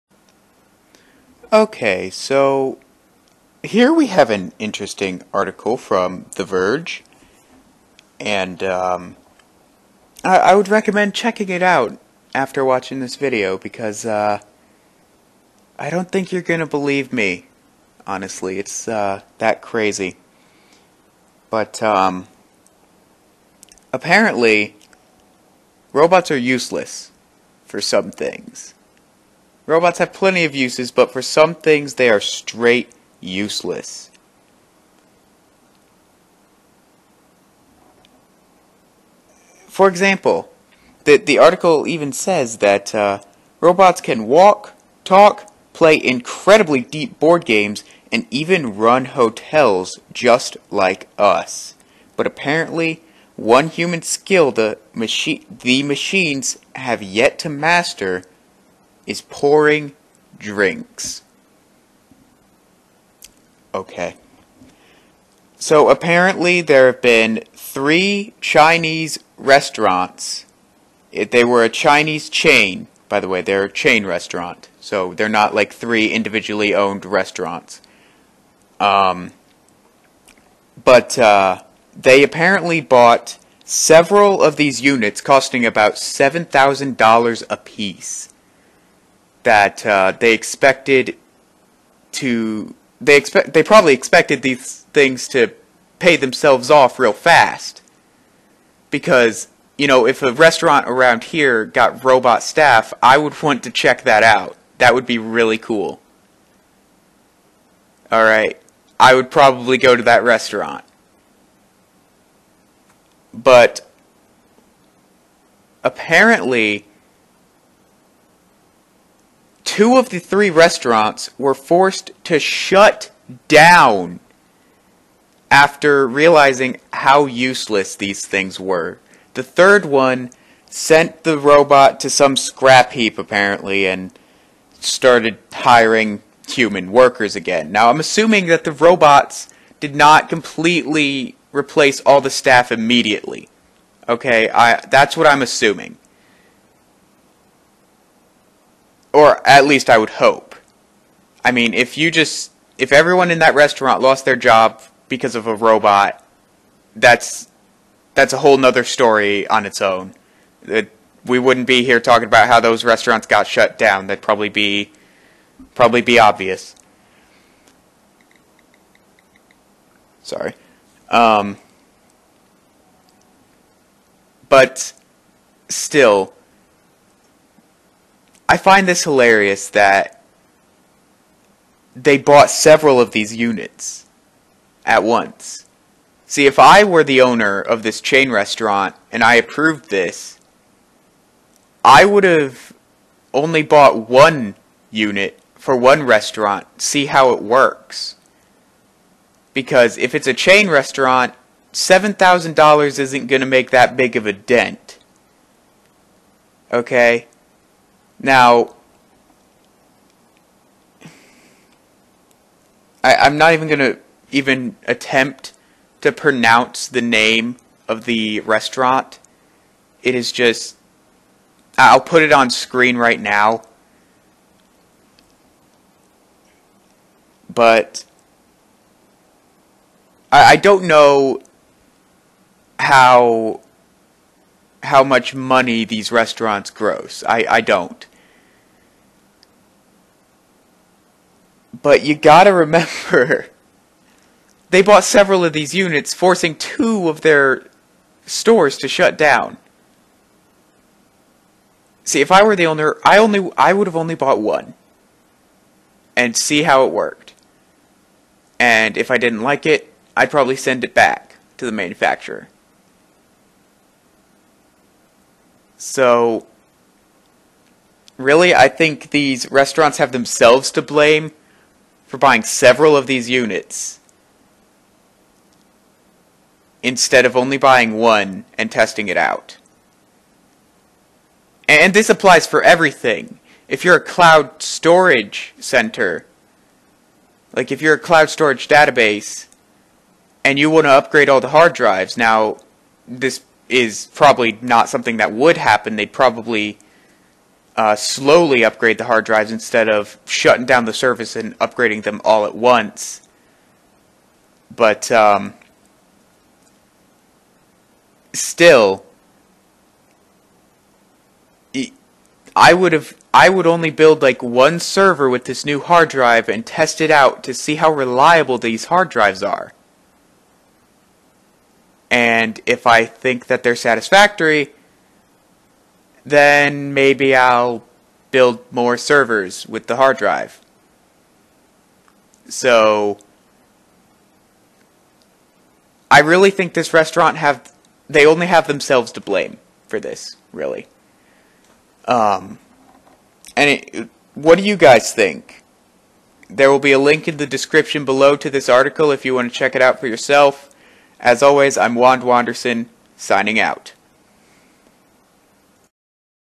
Useless Robots Force Two Chineese Restaurants to Shut Down
This is quite interesting. That's all I got.